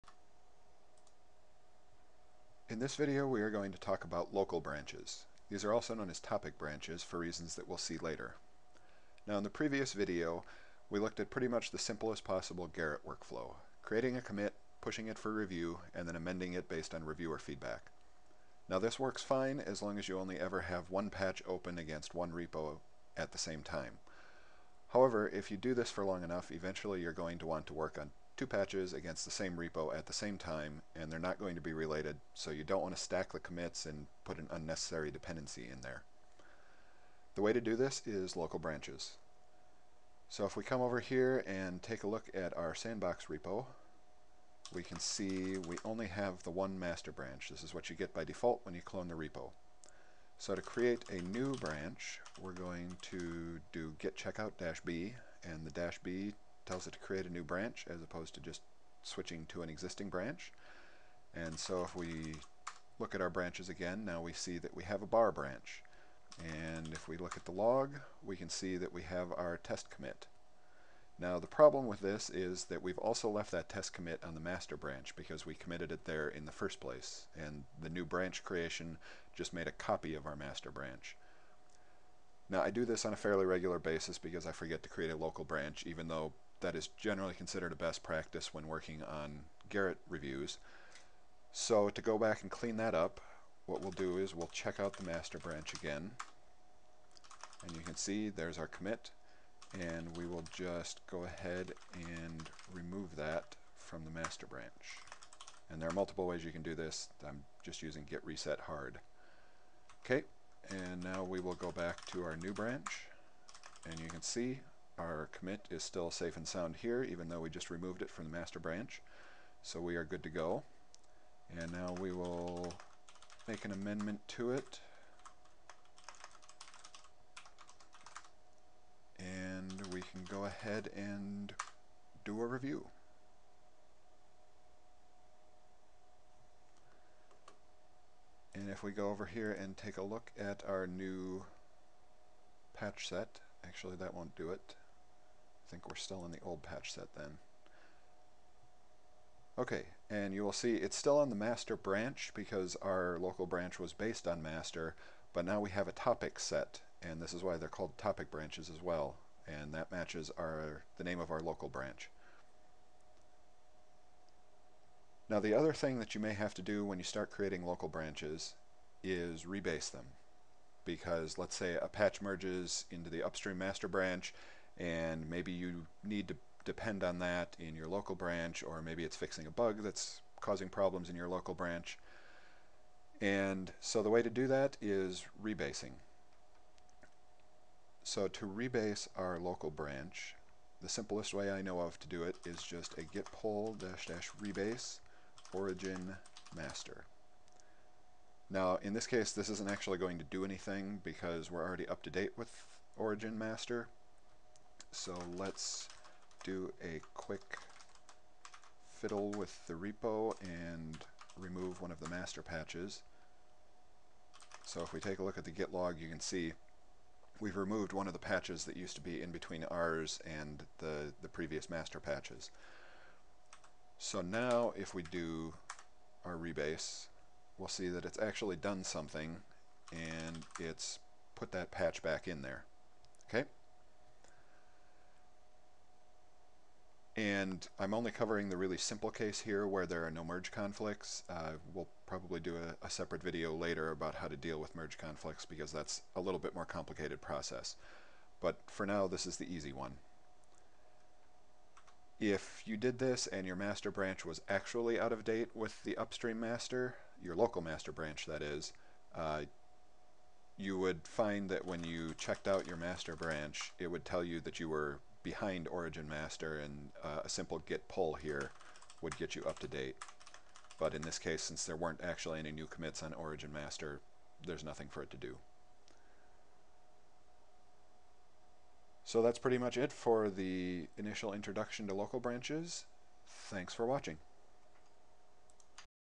Using Local Branches
A demonstration of how to use local branches when doing development in a Gerrit review environment.